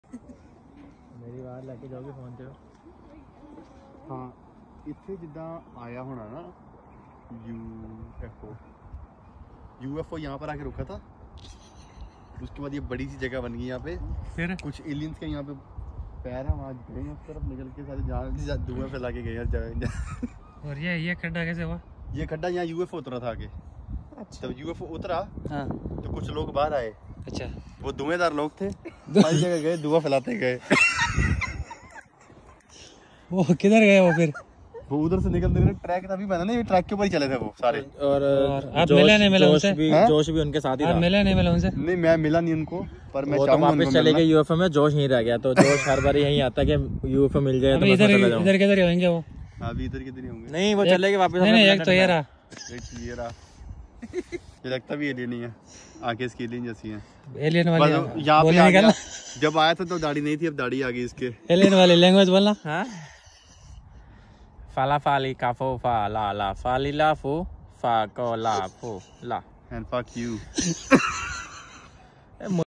frist vlog after reaching New Zealand 🇳🇿 |with some best funny friends At geothermal site in taupo
This is my first video shoot with my new iPhone 14

Location- Taupo New Zealand

Category- funny and vloging